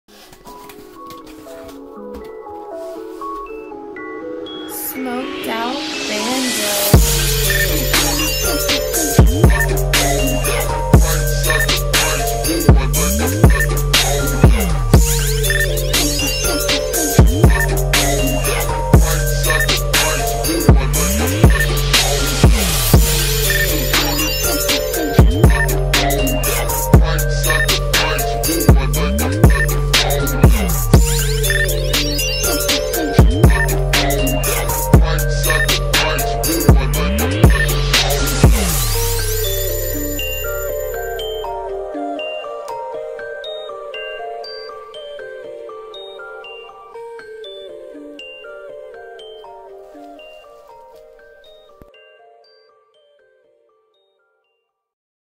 Sakura D4 Unbox
Finally decided to get back into rc drifting after about a year break.